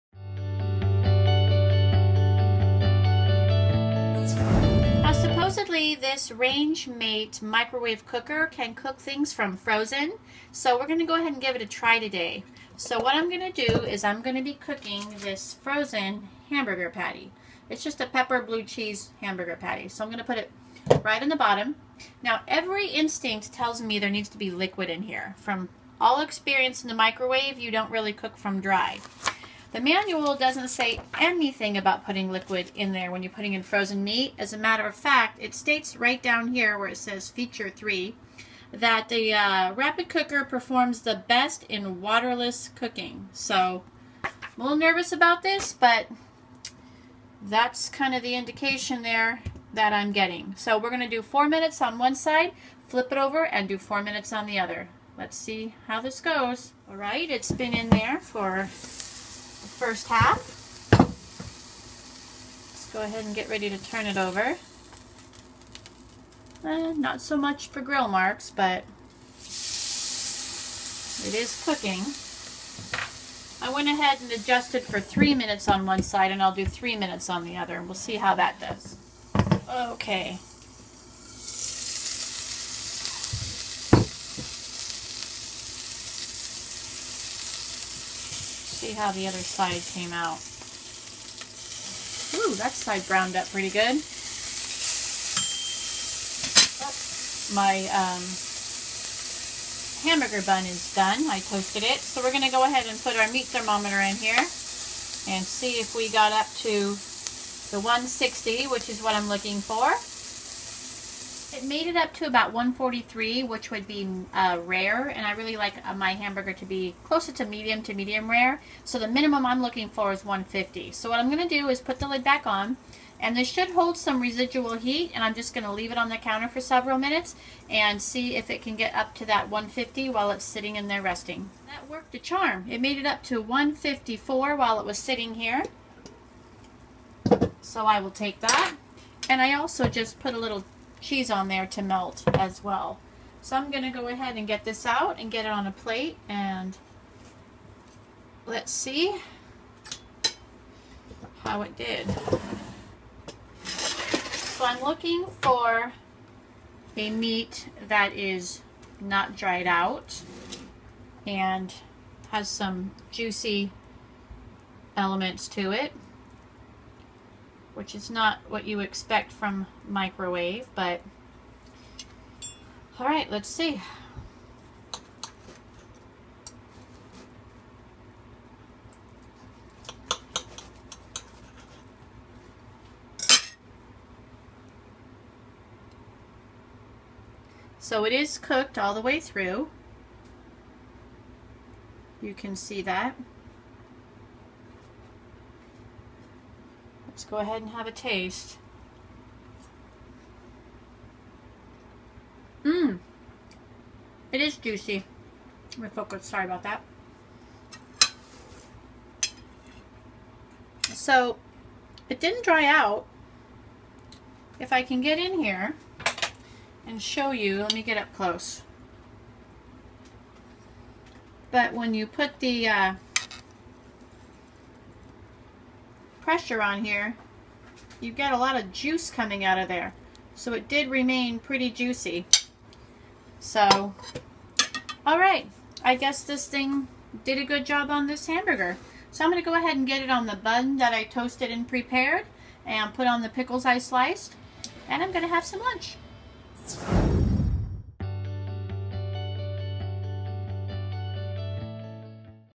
Range Mate Burger
I put the Range Mate to the test with a frozen hamburger patty. Can it handle it or will I get a dry hockey puck?